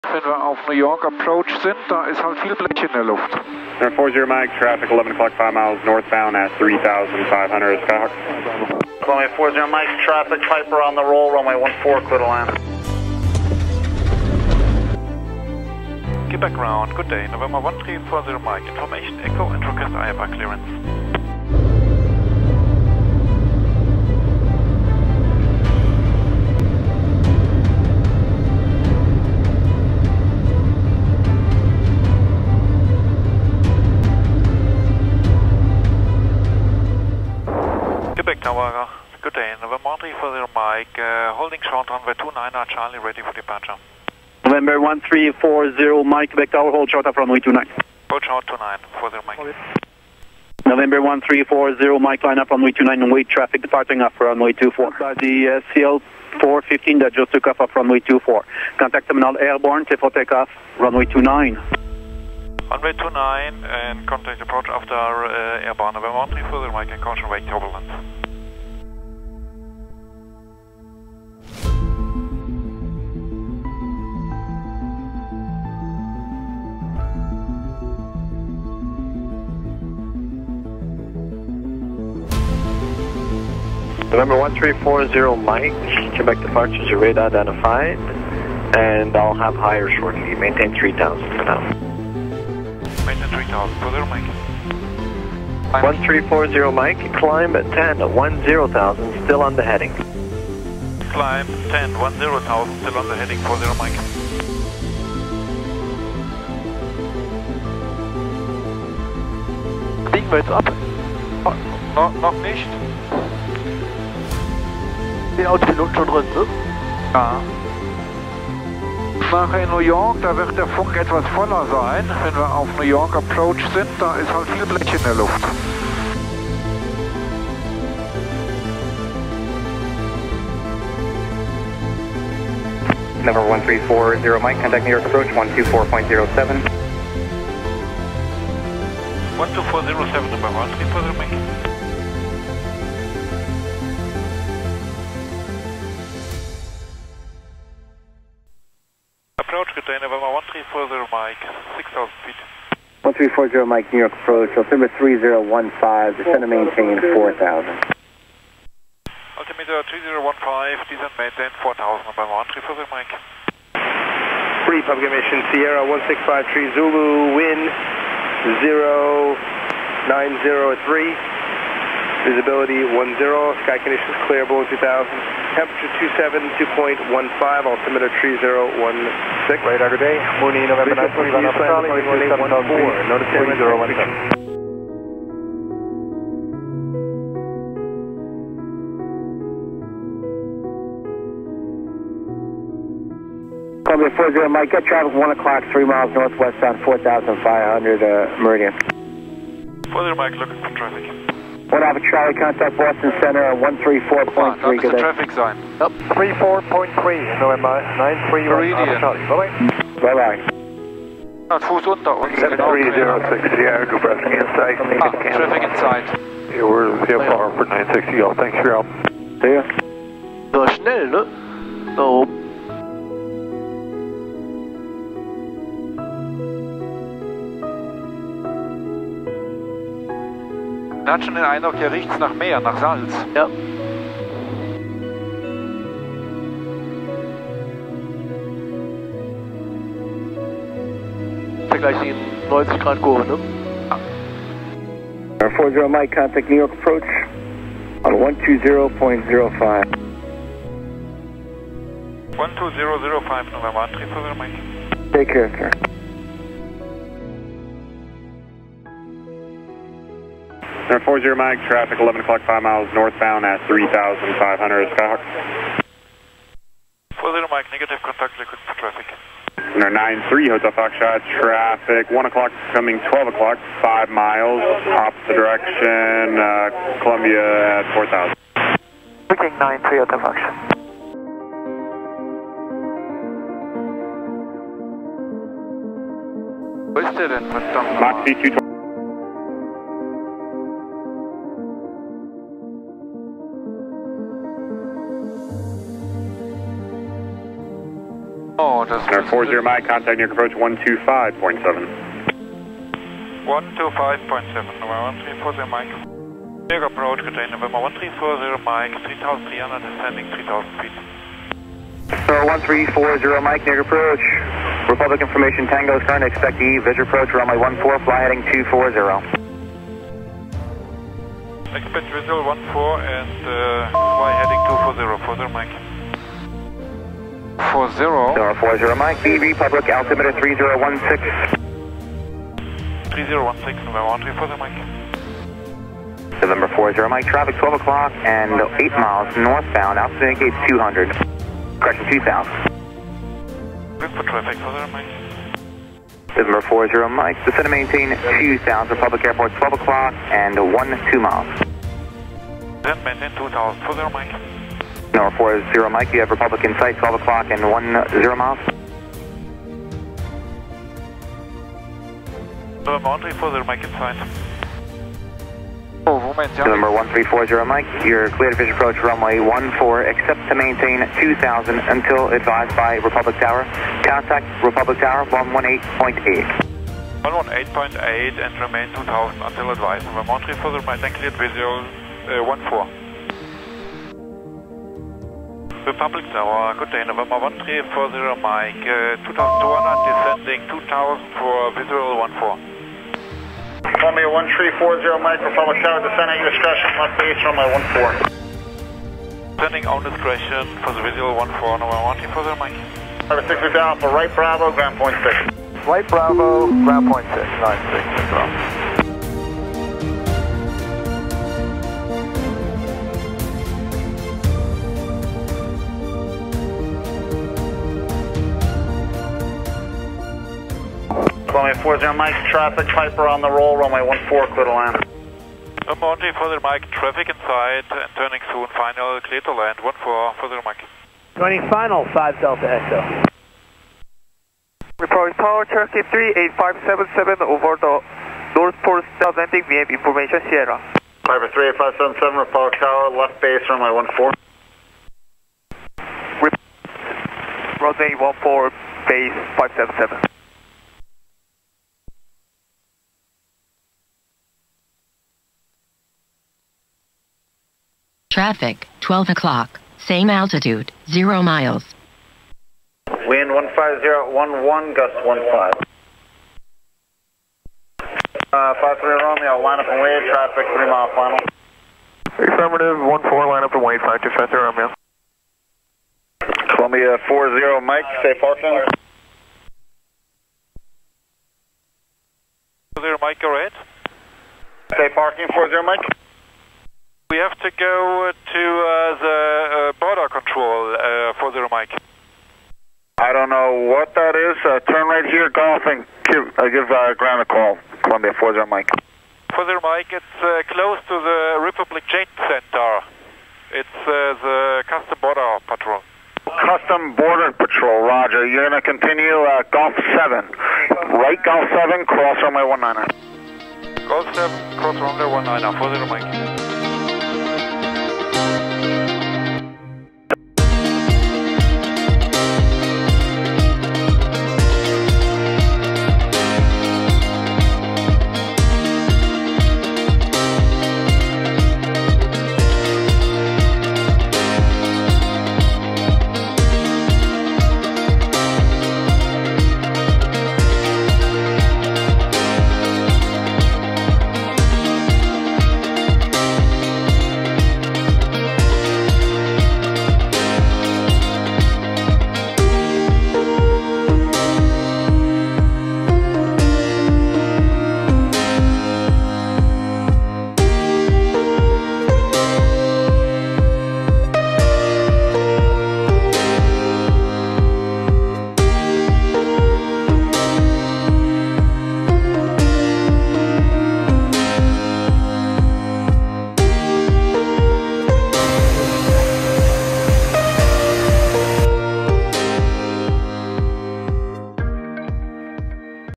Flug von Québec nach New York - voller Luftraum
Von Deutschland mit dem eigenen Flieger in die USA zum größten Fliegertreffen nach Oshkosh. Gemeinsam mit anderen Piloten aus der Pilot und Flugzeug Gruppe.

Einreise in die USA aus Canada. Das muss langfristig geplant sein, alleine das B1 Visum das notwendig ist kann einige Monate dauern. Nach der Landung muss man direkt zum Zoll rollen, darf auch nicht aussteigen bevor dieser kommt. Das ganze wird natürlich im Vorfeld angemeldet und nochmals telefonisch mit der Landezeit bestätigt. Um New York ist viel in der Luft unterwegs, es hagelt nur so Verkehrsmeldungen.